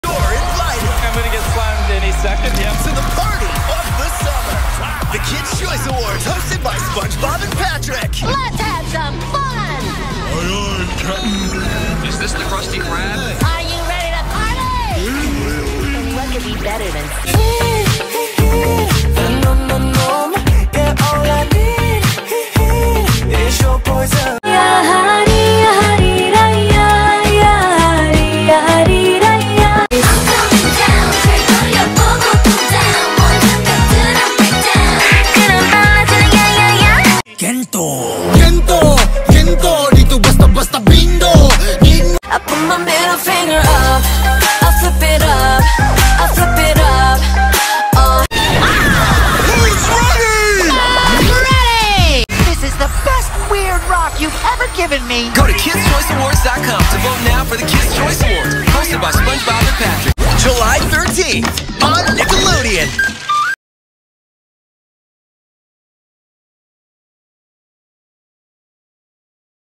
nickelodeon Kids' Choice Awards 2024 「FAV ASIAN ACT」Nominees Announcement
In this video, we made an informative announcement displaying the lineup of nominees for the Kid's Choice Awards 2024. You will learn how to vote for your favorite Asian act and when this event will be held.